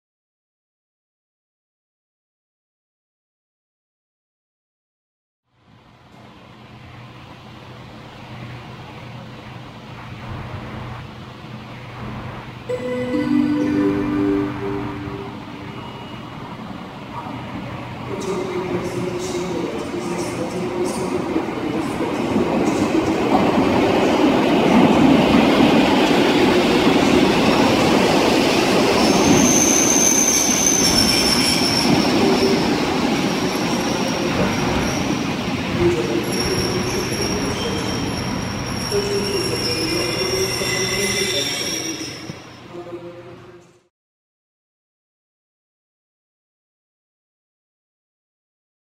EN57-1192 jako R 67322/3 Wilczka do Poznania Głównego.
Regio 67322/3 Wilczka z Jeleniej Góry do Poznania Głównego na odcinku Jelenia Góra-Wrocław Główny jechała w postaci EN57-1192. Na stacji Wrocław Główny podstawiono już skład wagonowy z lokomotywą, która pociągnęła skład do Poznania Głównego.

Województwo: Dolnośląskie
Miejscowość: Wrocław
Stacja: Wrocław Główny
Data: 23 Luty 2016r.
Godzina: 08:20
Przewoźnik: Przewozy Regionalne